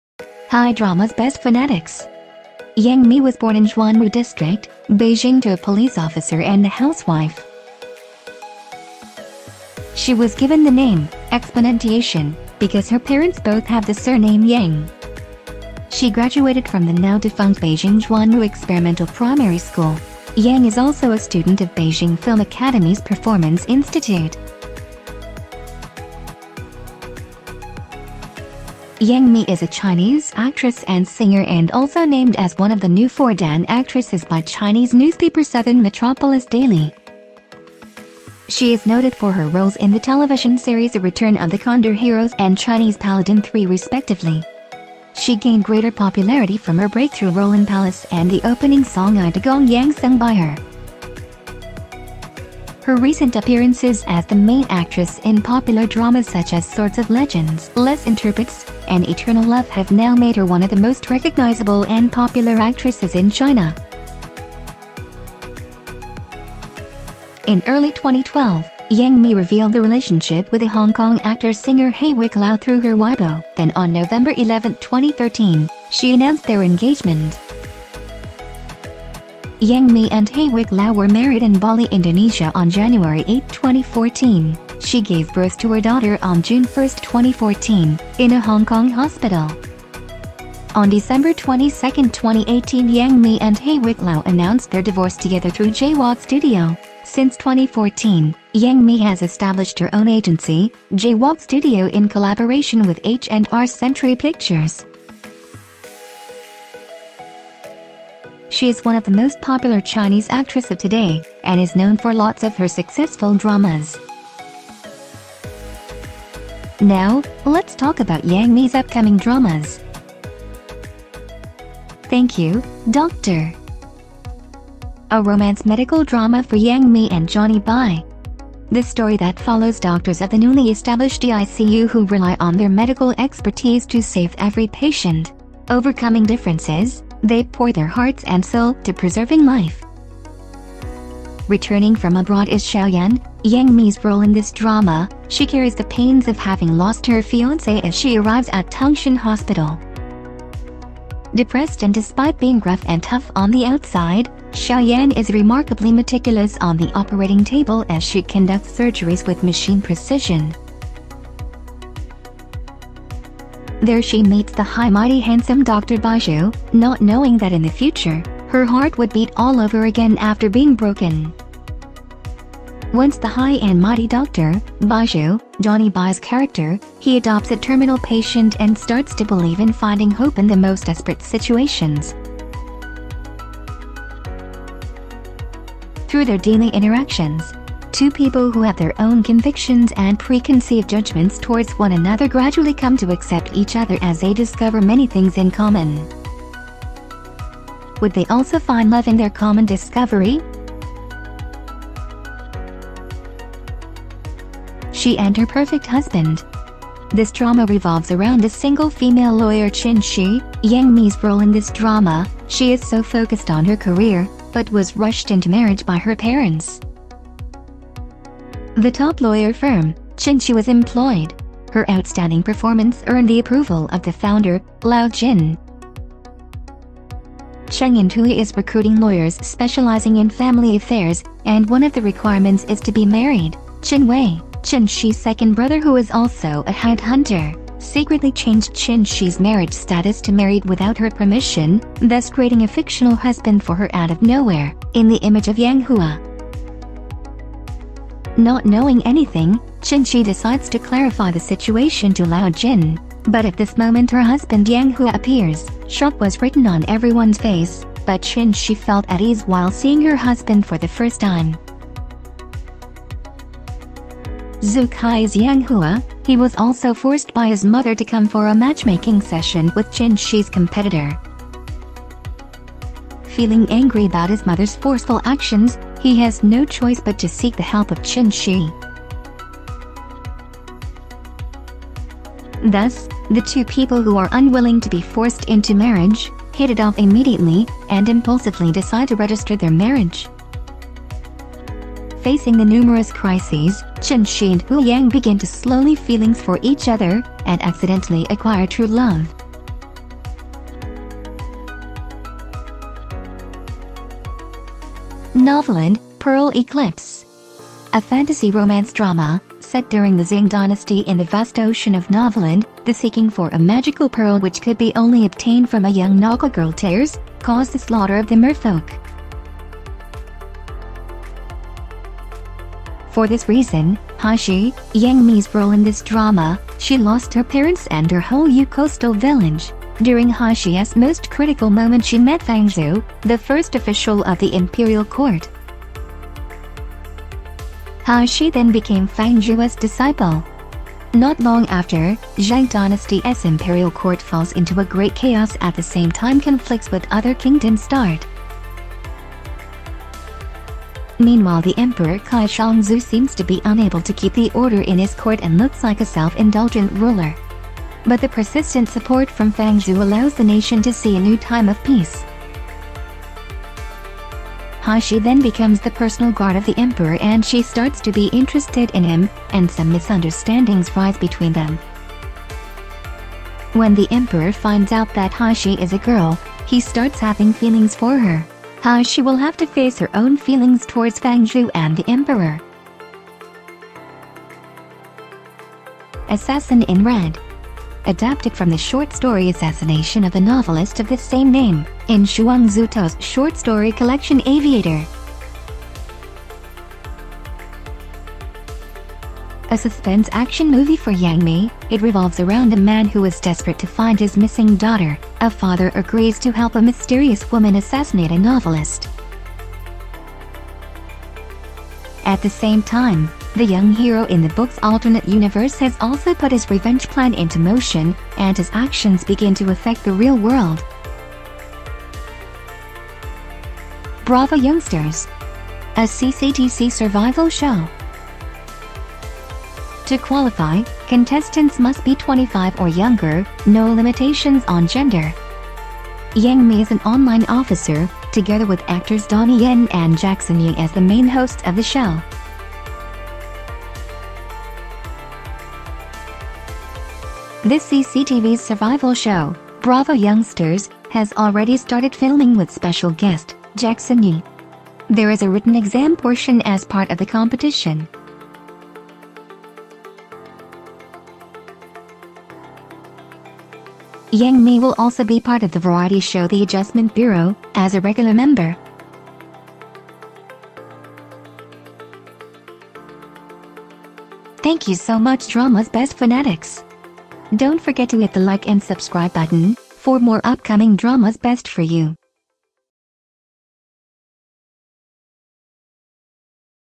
Watch out for Yang Mi's Dramas for 2021 and 2022
All about Actress Yang Mi's updated Dramas to watch on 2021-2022
Lists are the Confirmed Dramas

DRAMAS:
Thank You, Doctor
She and Her Perfect Husband
Novoland: Pearl Eclipse

MOVIE:
Assassin in Red

Sources: Dramawiki/Dramalist

Thank you so much Dramas Best Fanatics!
Don't forget to support Dramas Best Channel for more updates!